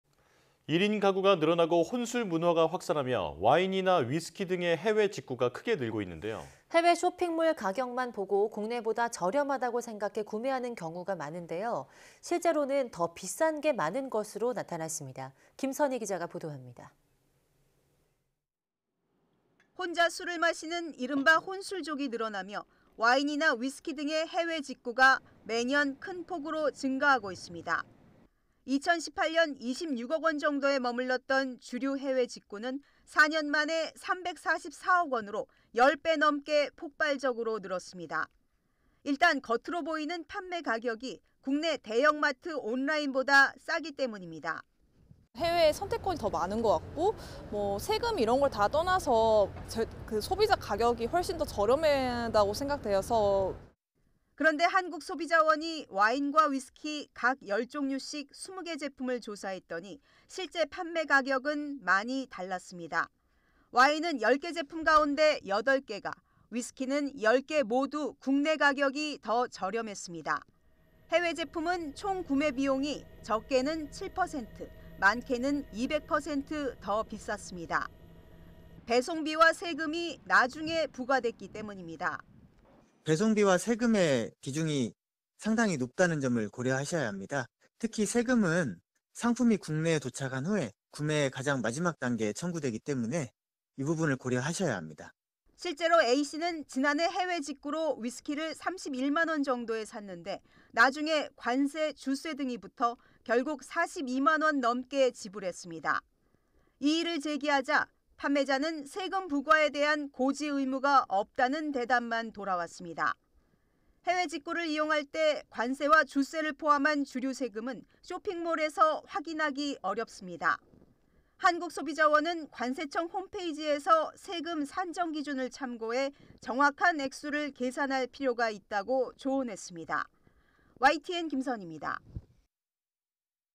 와인·위스키, 해외직구보다 국내서 사는 게 더 싸다 / YTN 사이언스
[앵커]
1인 가구가 늘어나고 혼술 문화가 확산하며 와인이나 위스키 등의 해외 직구가 크게 늘고 있는데요.

해외 쇼핑몰 가격만 보고 국내보다 저렴하다고 생각해 구매하는 경우가 많은데 실제로는 더 비싼 게 많은 것으로 나타났습니다.

김선희 기자가 보도합니다.

[기자]
혼자 술을 마시는 이른바 '혼술 족'이 늘어나며 와인이나 위스키 등의 해외 직구가 매년 큰 폭으로 증가하고 있습니다.

2018년 26억 원 정도에 머물렀던 주류 해외 직구는 4년 만에 344억 원으로, 10배 넘게 폭발적으로 늘었습니다.

일단 겉으로 보이는 판매가격이 국내 대형마트 온라인보다 싸기 때문입니다.

[양나원 / 서울 등촌동 : 해외에 선택권이 더 많은 것 같고 세금을 떠나서 소비자 가격이 훨씬 더 저렴하다고 생각이 돼서….]

그런데 한국소비자원이 와인과 위스키 각 10종류씩 20개 제품을 조사했더니 실제 판매가격은 많이 달랐습니다.

와인은 10개 제품 가운데 8개가, 위스키는 10개 모두 국내 가격이 더 저렴했습니다.

해외제품은 대부분 총구매비용이 적게는 7% 많게는 200% 더 비쌌습니다.

배송비와 세금이 나중에 부과됐기 때문입니다.

[이후정 / ...